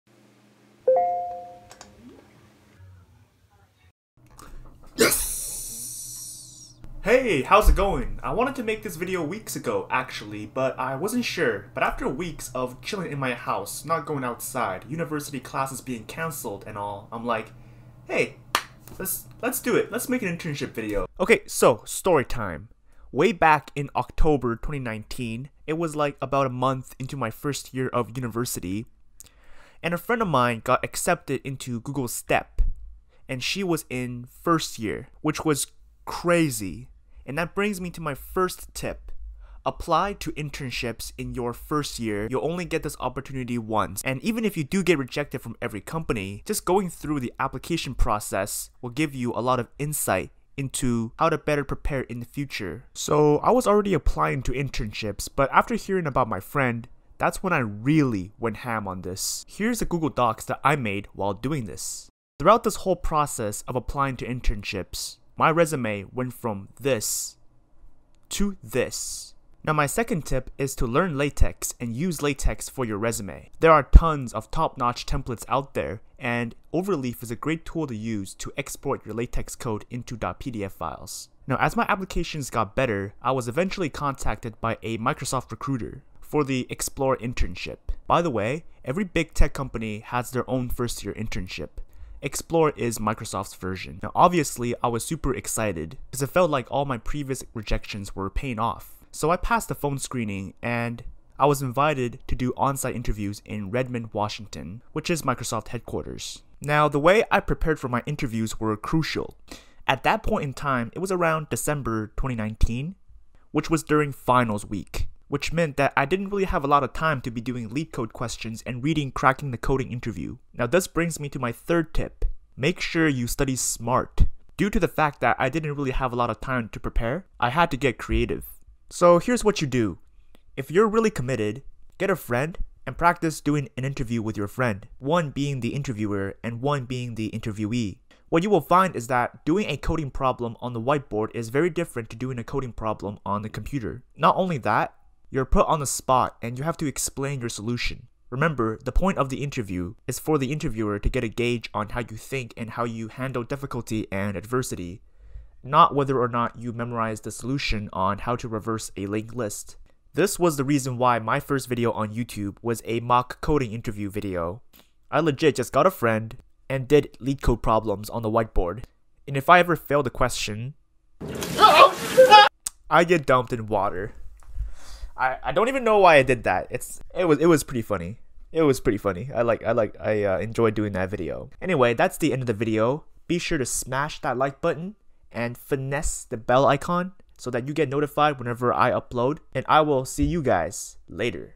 How I Got a Software Engineering Internship at Microsoft in 1st Year University
► My Super low-budget camera
► The book I am currently reading